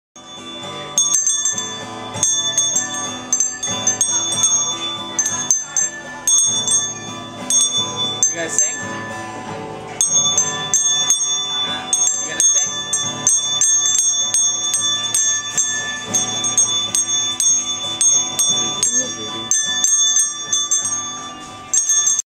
Abbie's Concert on Thanksgiving.wmv
Abbie's concert at Cracker Barrel on Thanksgiving